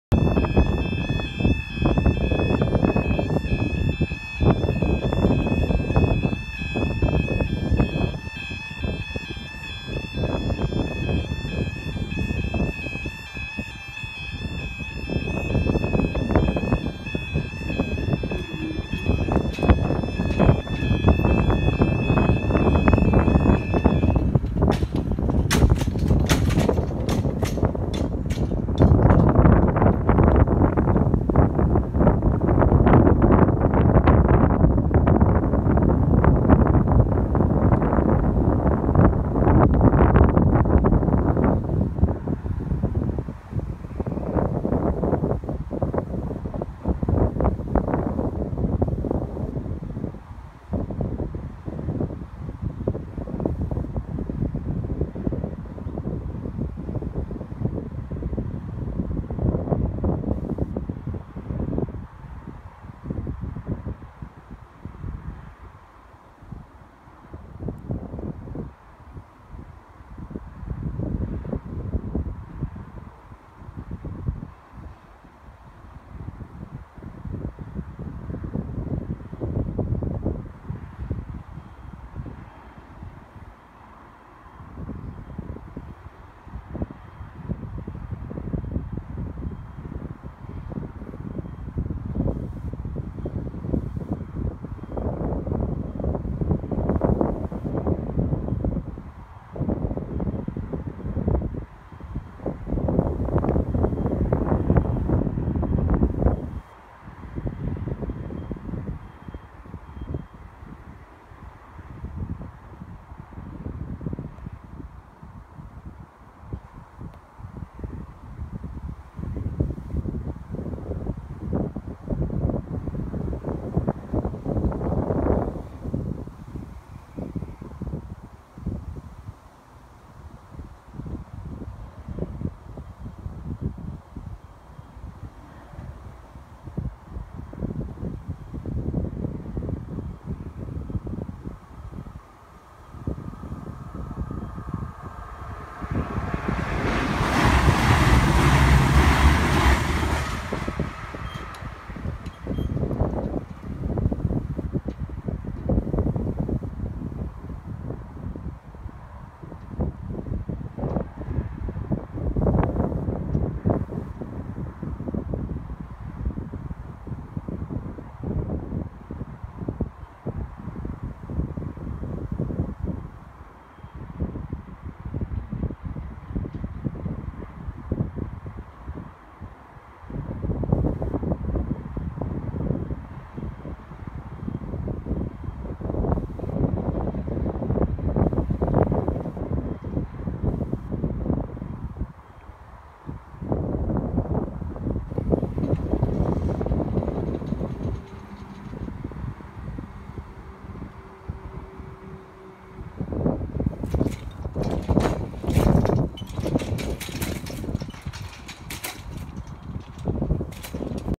Hewish LC, Hewish, Somerset
Again, barriers down for a full minute after train had passed. Not 100% sure why.